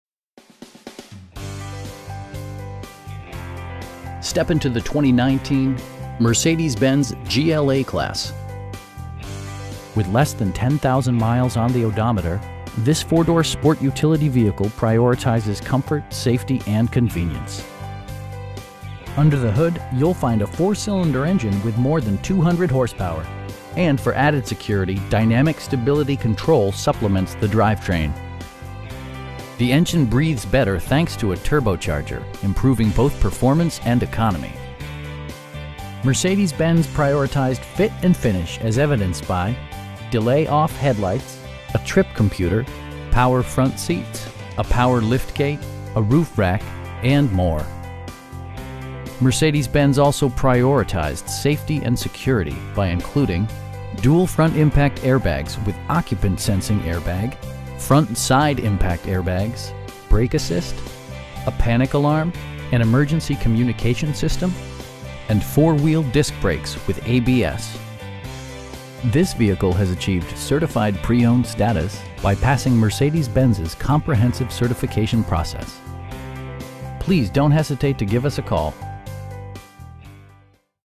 2019 Mercedes-Benz GLA 250 4MATIC in New London, CT 06320
Mercedes-Benz of New London

Step into the 2019 Mercedes-Benz GLA-Class. With less than 10,000 miles on the odometer, this 4 door sport utility vehicle prioritizes comfort, safety and convenience. Under the hood you'll find a 4 cylinder engine with more than 200 horsepower, and for added security, dynamic Stability Control supplements the drivetrain. The engine breathes better thanks to a turbocharger, improving both performance and economy. Mercedes-Benz prioritized fit and finish as evidenced by: delay-off headlights, a trip computer, power front seats, tilt steering wheel, a power liftgate, a roof rack, and more. Mercedes-Benz also prioritized safety and security by including: dual front impact airbags with occupant sensing airbag, front side impact airbags, traction control, brake assist, a panic alarm, an emergency communication system, and 4 wheel disc brakes with ABS. You'll never lose visibility with rain sensing wipers, which activate automatically when the drops start to fall. This vehicle has achieved Certified Pre-Owned status, by passing Mercedes-Benz's comprehensive certification process. Our aim is to provide our customers with the best prices and service at all times. Please don't hesitate to give us a call.